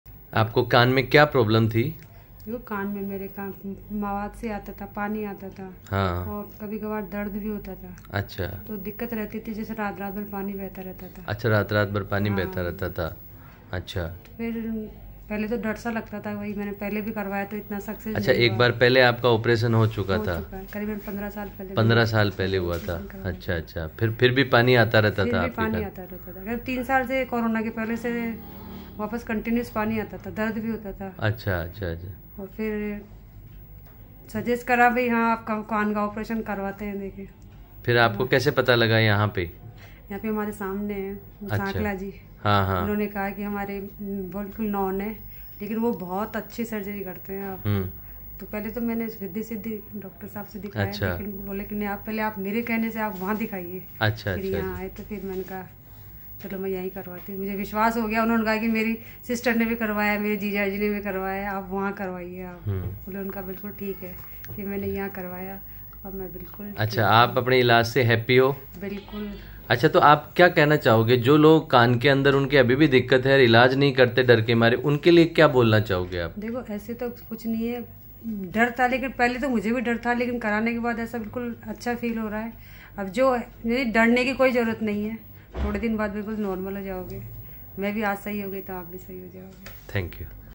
Happy Patient After Ear Surgery - Jaipur Doorbeen Hospital
Another Happy Patient of Dr Sunil Tanvar.- Check the complete video for his overall experience of rhinoplasty . Consult with Dr Sunil Tanvar (Rhinoplasty Surgeon & ENT Expert) for more information - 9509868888

Visit: Jaipur Doorbeen Hospital